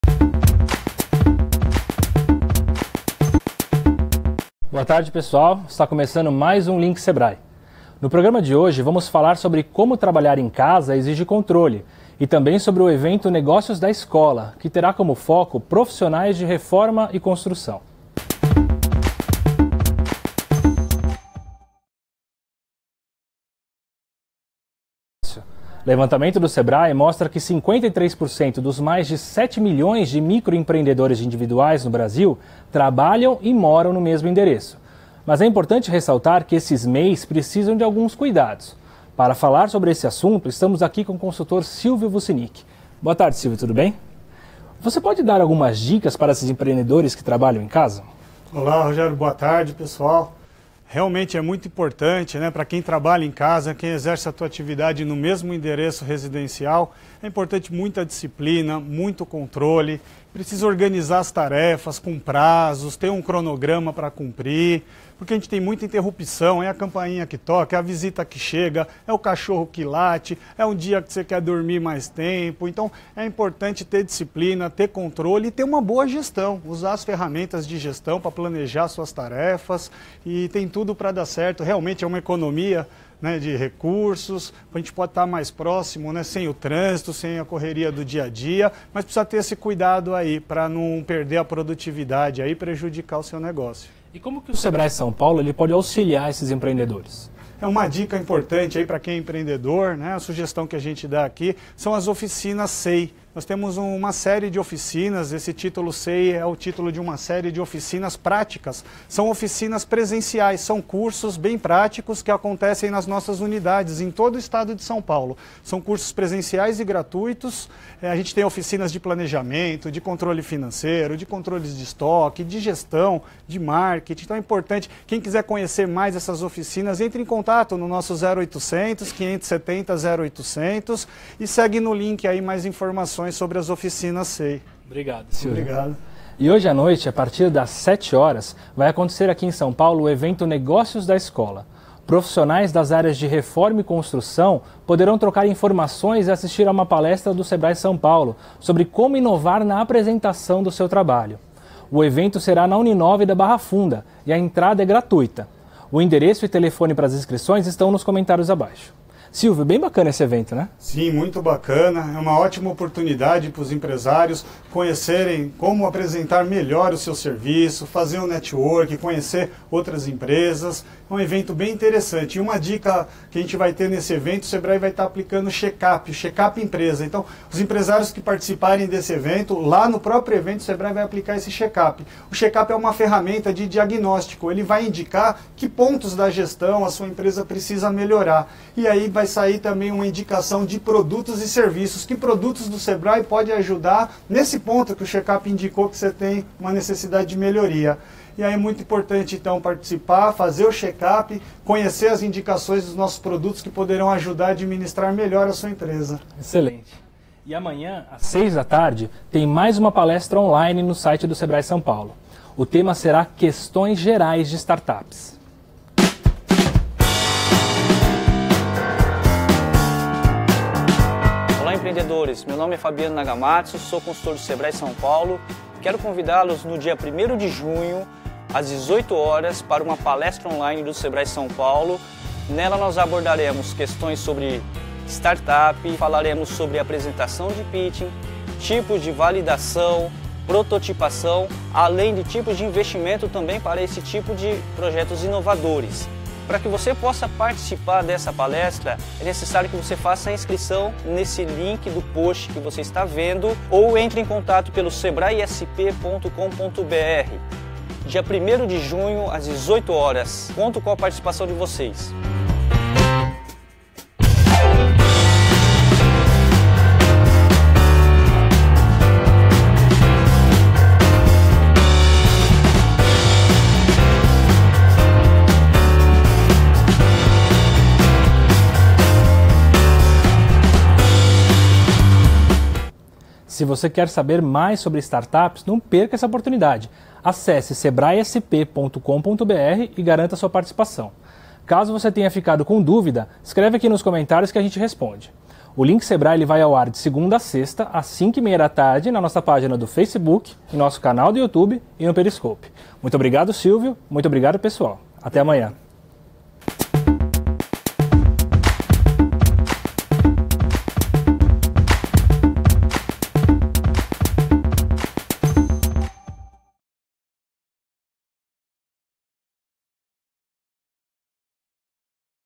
Link Sebrae - Trabalhar em casa exige controle e desempenho
Começa mais um Link Sebrae, um resumo das notícias do dia que interessam ao empreendedor, com as soluções do Sebrae SP para ajudá-lo!

Pesquisa do Sebrae mostra que mais da metade dos MEIs formalizados no país trabalham e moram no mesmo endereço. Como ser mais produtivo?

De segunda a sexta, sempre às 17h30! 🕰️ Mande suas perguntas! 😉